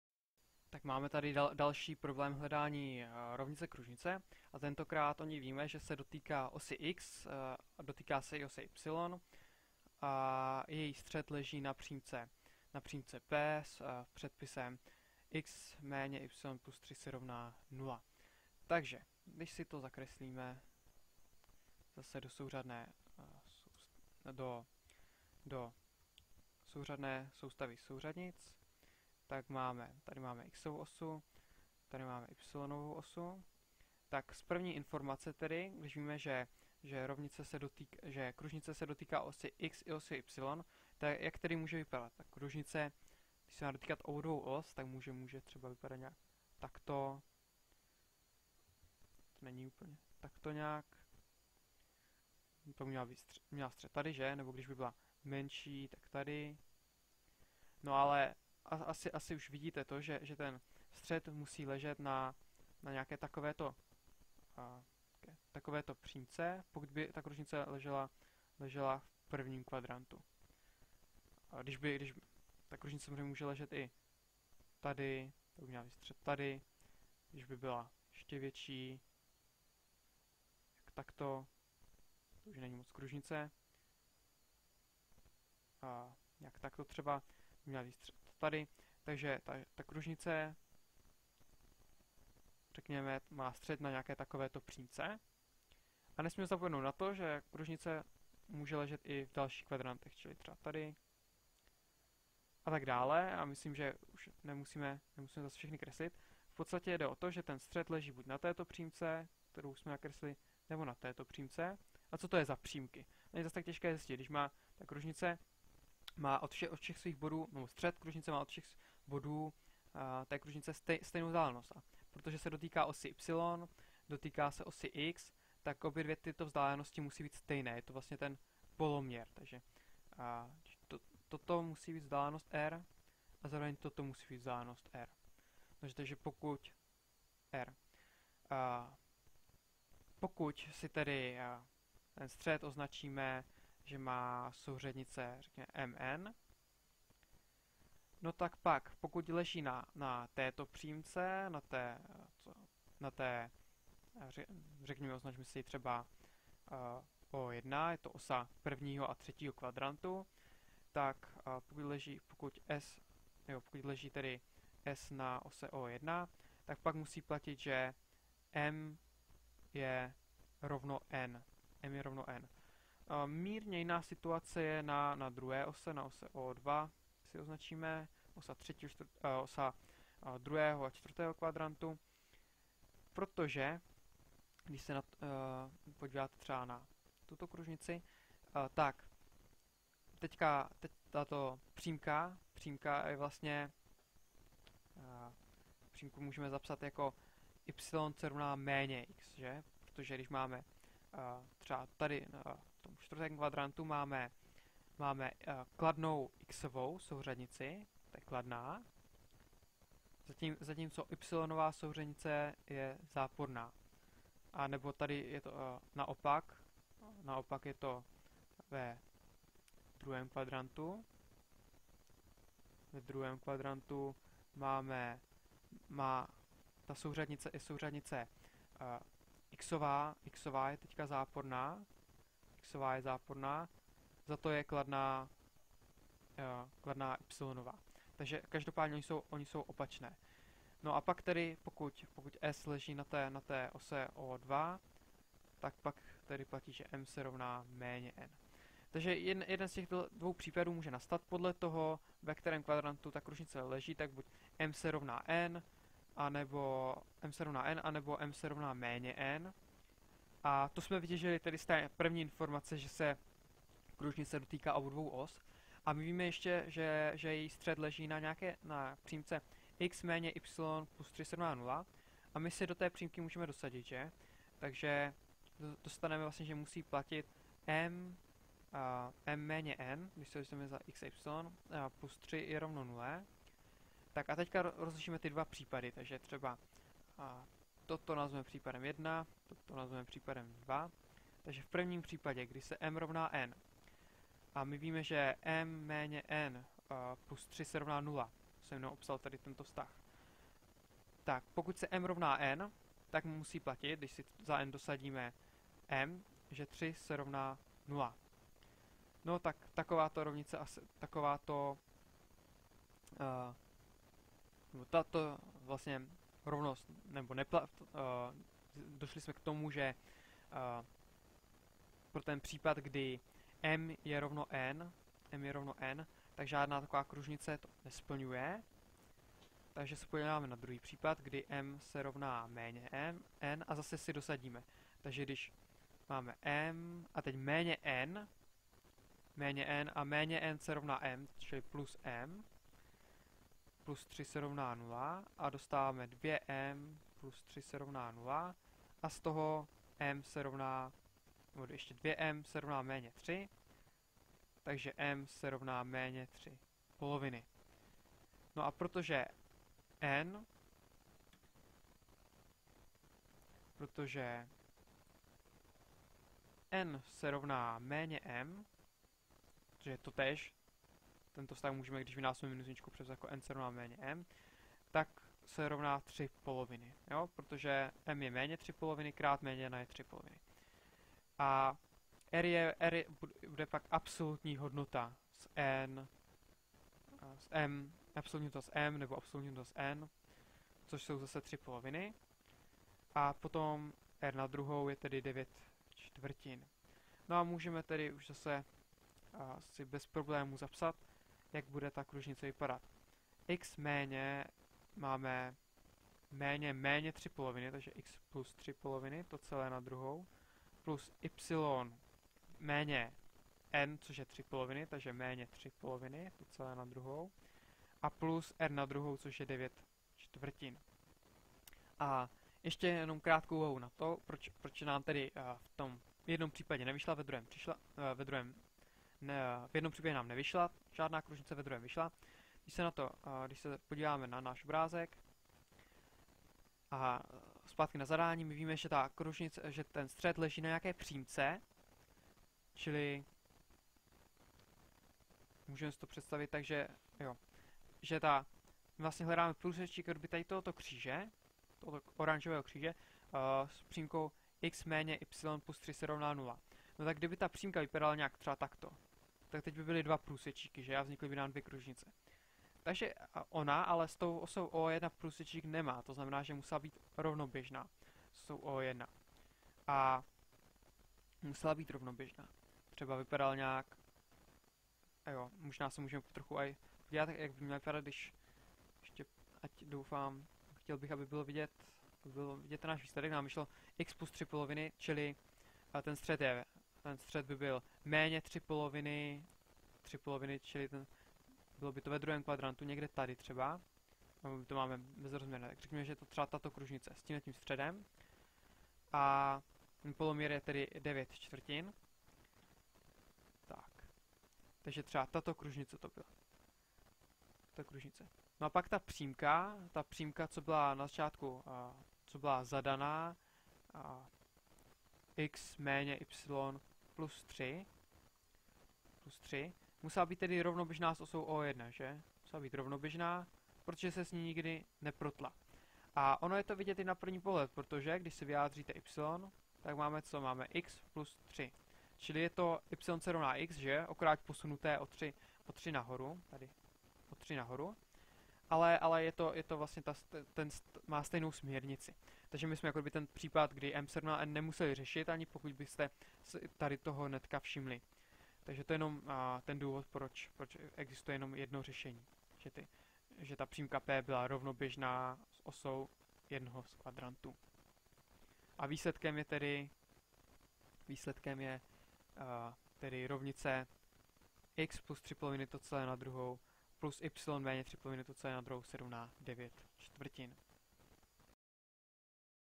Hledání rovnice kružnice 3
Zadání: Napište rovnici kružnice, která se dotýká osy x i osy y a její střed leží na přímce p: x - y + 3 = 0.